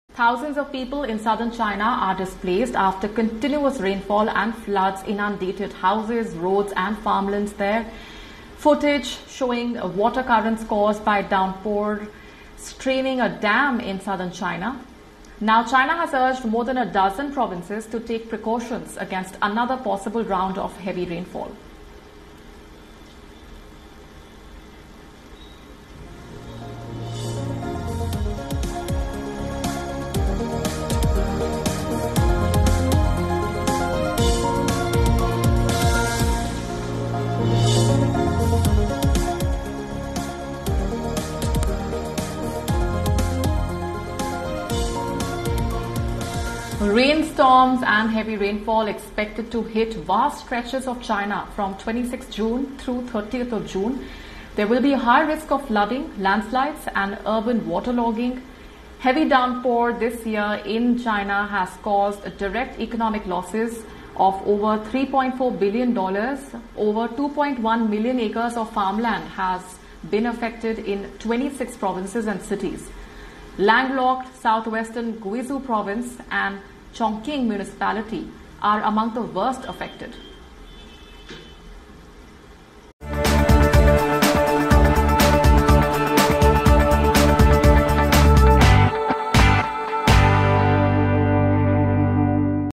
Heavy downpours trigger flash floods in large swathes of China
China has urged more than a dozen provinces to take precautions against another round of heavy rain in the next few days after turbulent weather brought floods to large swathes of the country this month.